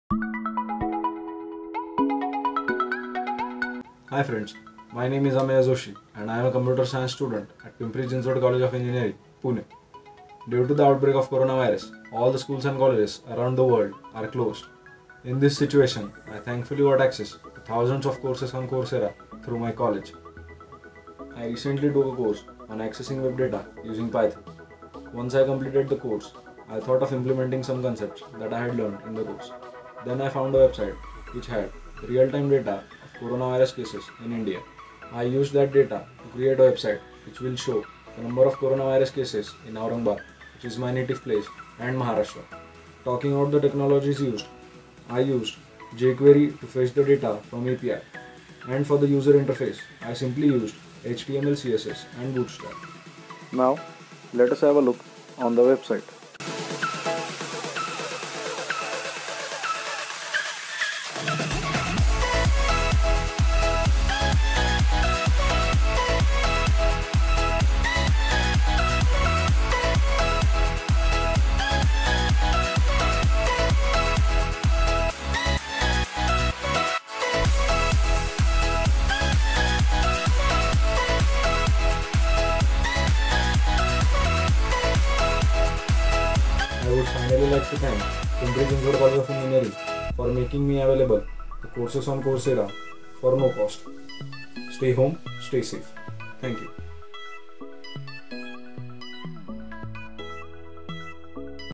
COVID-19 Tracker | Ameya Joshi, TE Computer, PCCOE
We at PCCOE always believe in Project Based Learning. One of our Third Year Computer Engineering student, Mr. Ameya Joshi, developed a web application to display real-time COVID19 cases in Aurangabad & Maharashtra region.